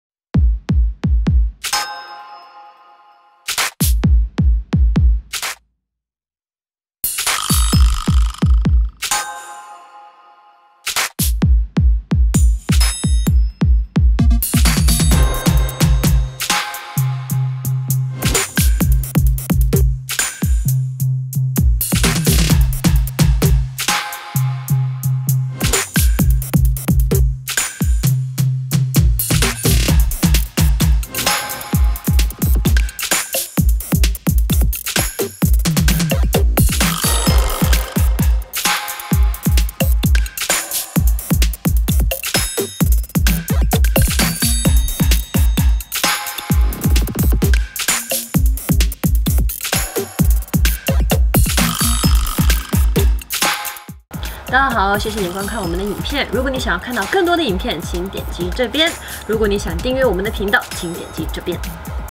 Get turned on... Feat. Carrie Wong (on the cover of 8 days)
Treat your eyes with this behind the scenes of the beautiful Carrie Wong! You're welcome...